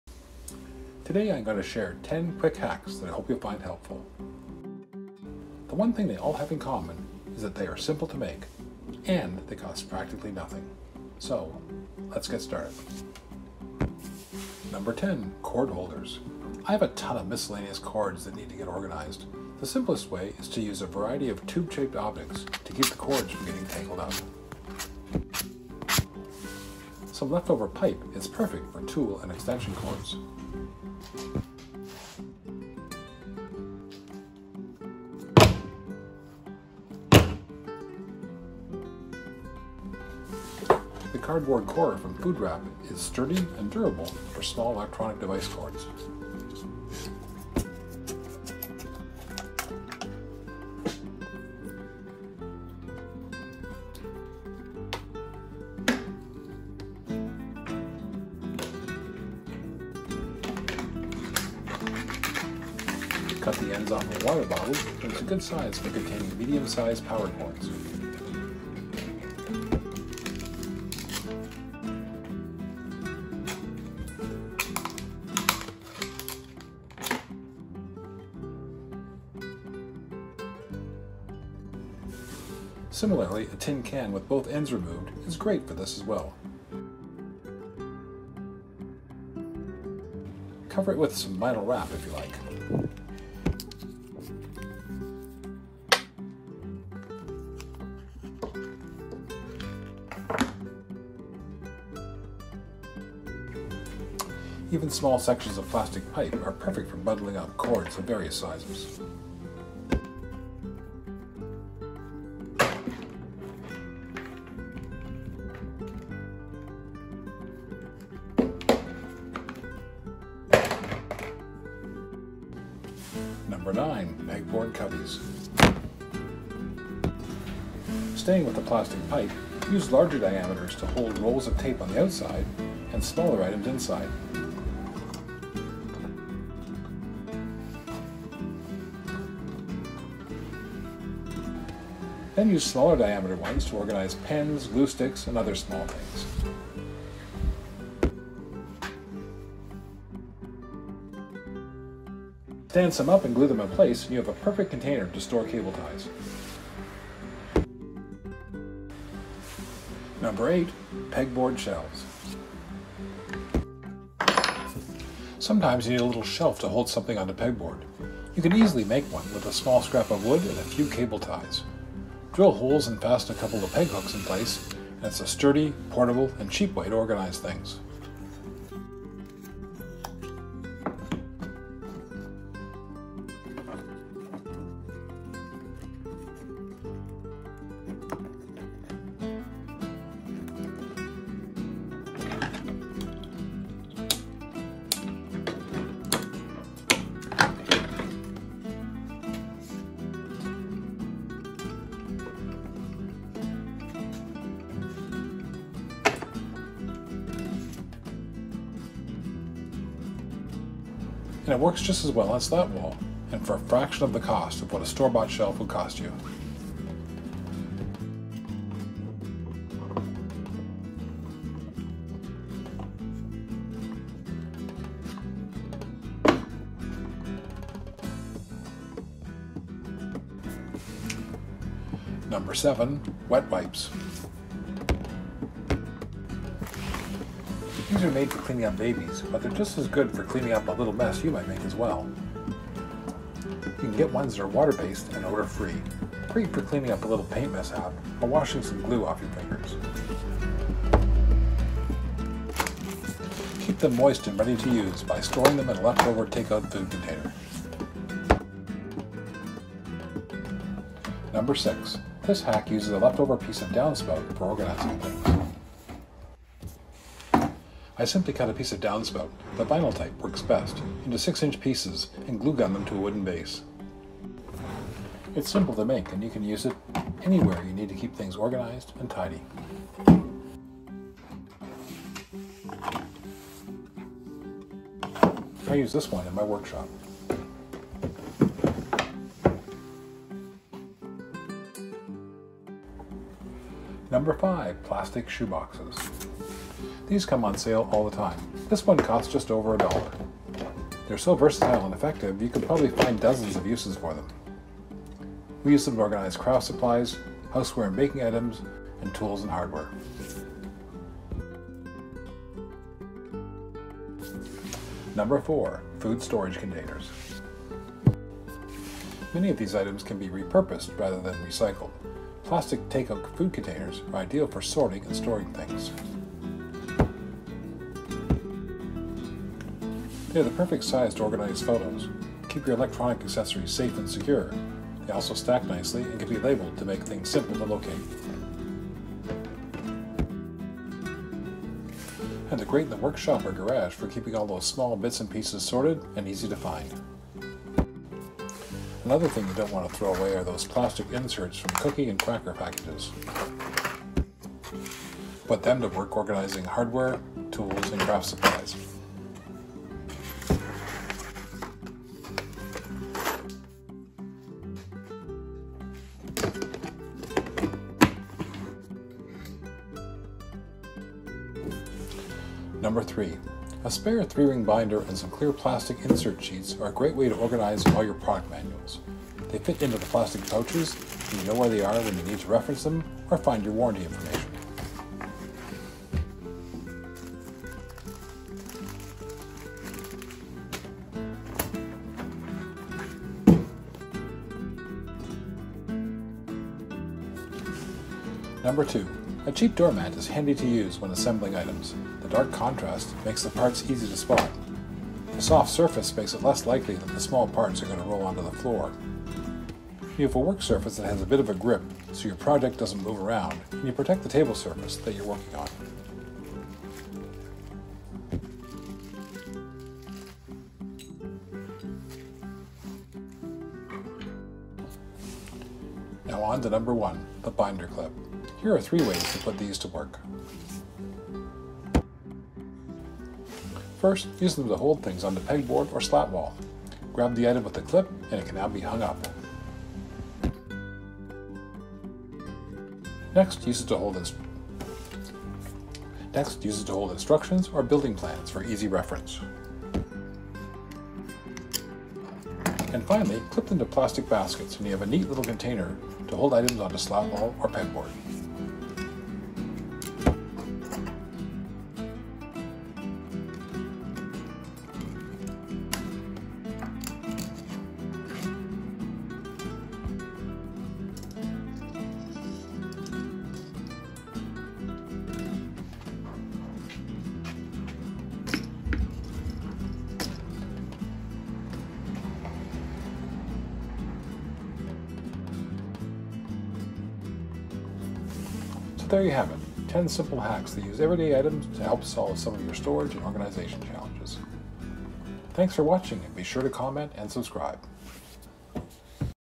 Amazing Home Hacks! 10 CHEAP & SIMPLE life hacks in less than 10 minutes!
Home Hacks, Workshop Hacks! Organize & simplify your life with 10 easy life hacks that cost practically nothing. Reuse, recycle and upcycle simple household items into storage and organization solutions. Don’t put out your trash or recycling before watching this!

Renovate, Reorganize, Reuse, Refresh. I have recently downsized and am finding all sorts of creative ways to make improvement and add space simply and inexpensively!